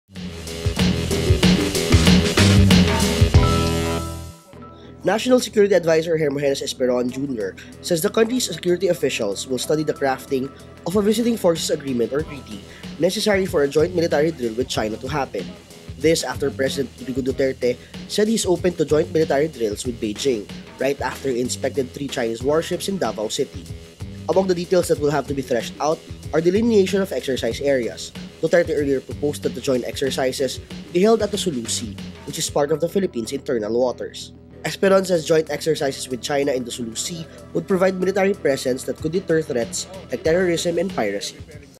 PH officials studying military drills with China
More videos on Rappler Follow Rappler for the latest news in the Philippines and across the globe. National Security Adviser Hermogenes Esperon Jr says PH security officials will study the crafting of a Visiting Forces Agreement or treaty necessary for a joint military drill with China to happen. Full story Follow Rappler for the latest news in the Philippines and around the world.